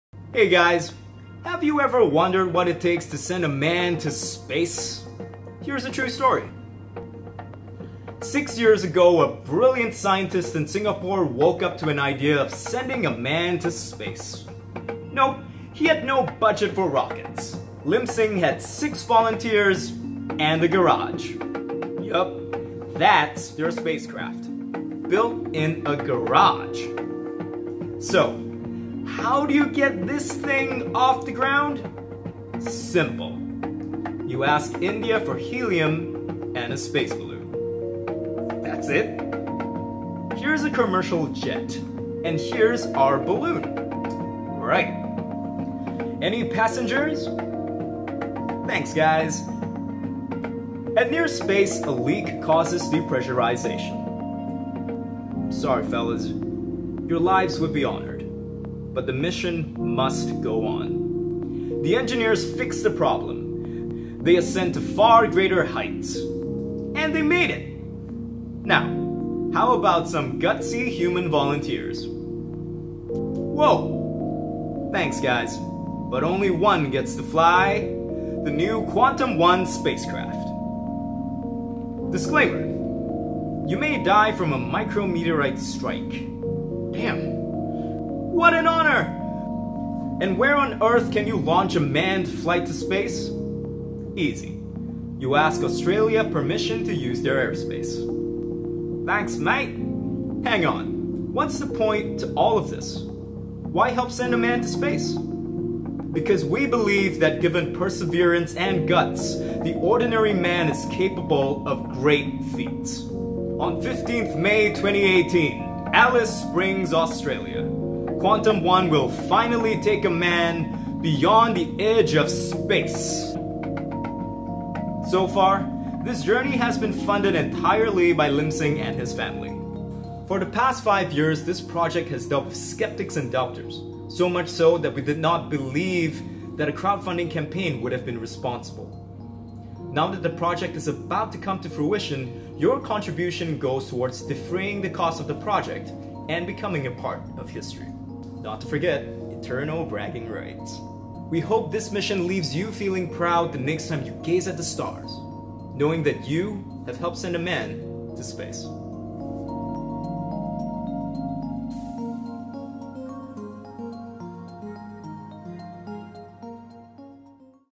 Help send the First Singaporean to Space
This project started with a quest to inspire not just Singaporeans but every ordinary person in any field of work, that given the heart, mind, and perseverance it is possible to dream and accomplish great things.

This is a story for you.